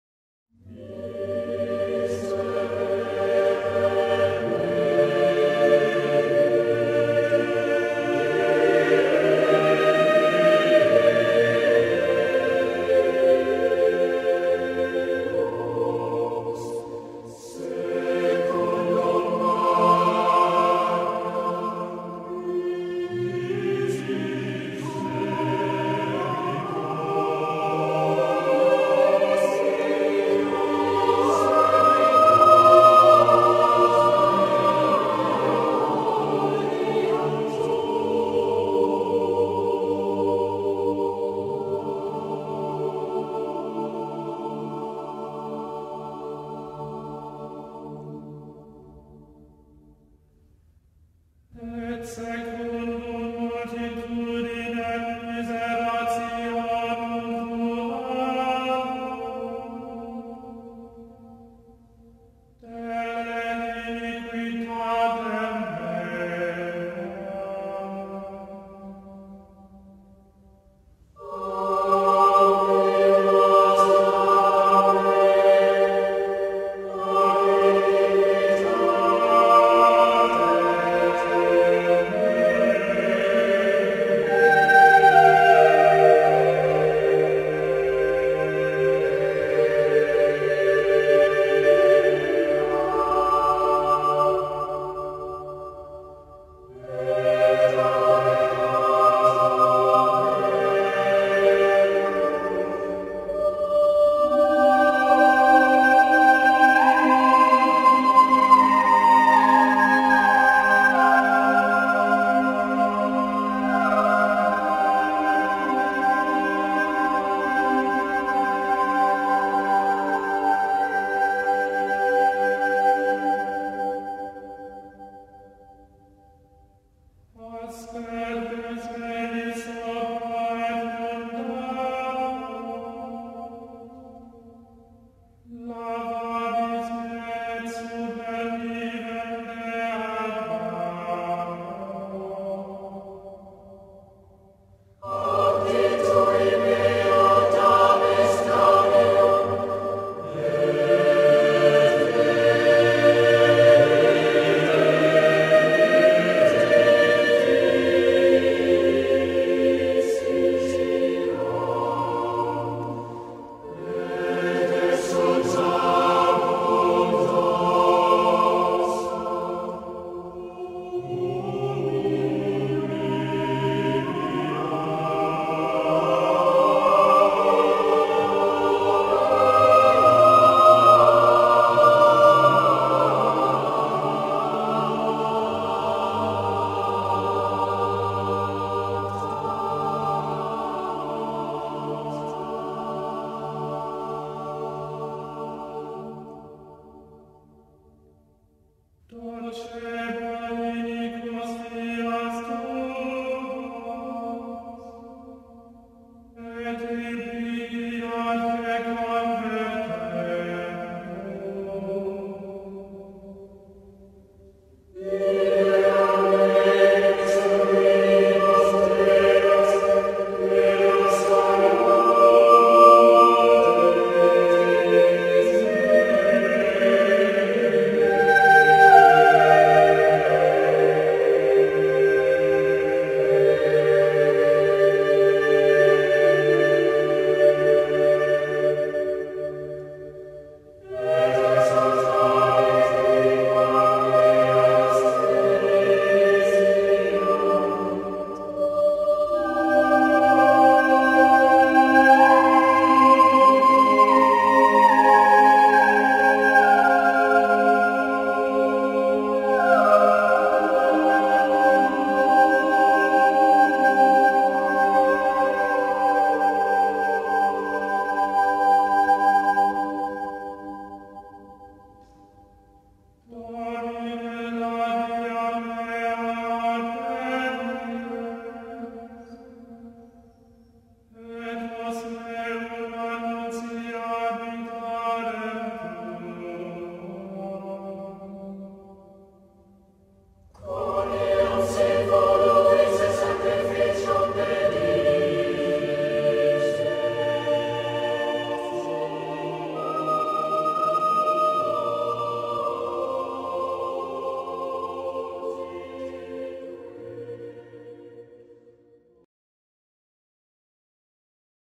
Lincoln Cathedral
This AV presentation of Lincoln Cathedral was photographed and shows the interior and exterior of this magnificence architectural structure.